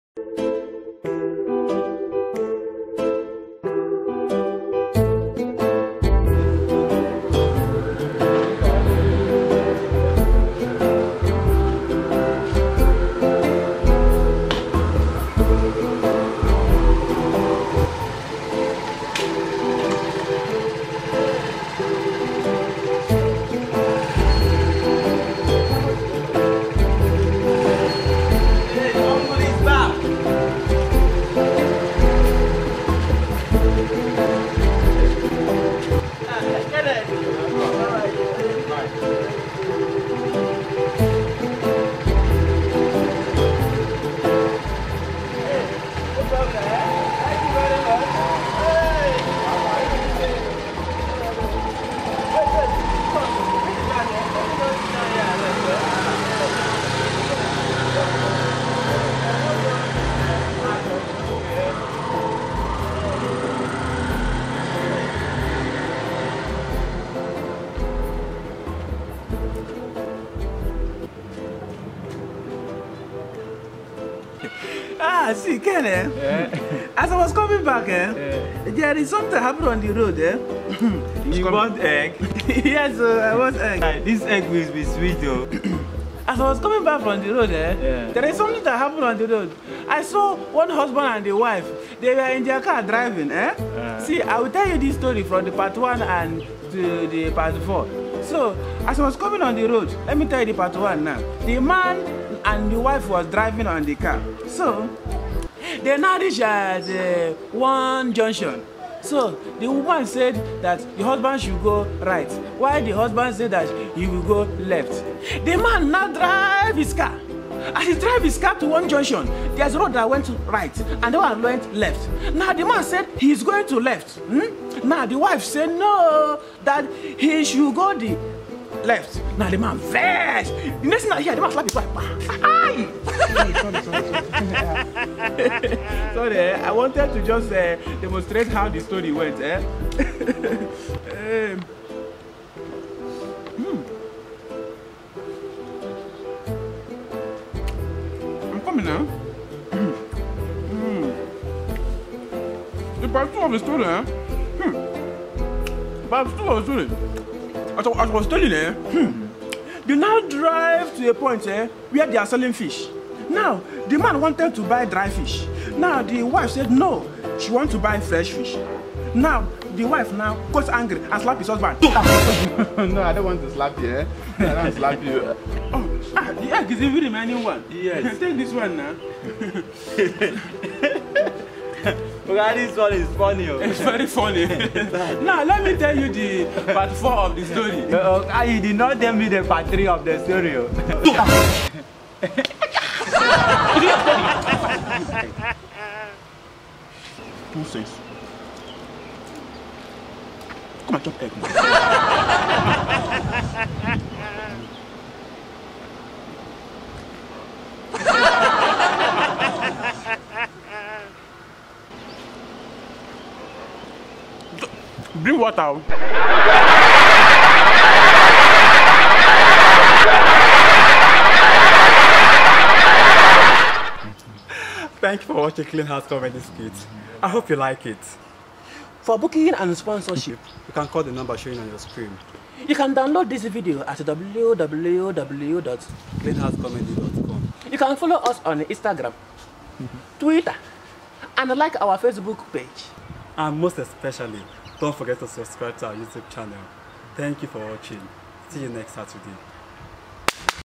YOUR STORY [Clean House Comedy] Episode 12
This is a very funny episode where Daddyhumble came back from his village with three egg and his boy Ken wanted to take the egg in a funny way. just watch  Daddyhumble funny reaction when he saw him taking the egg. please subscribe to our channel after watching.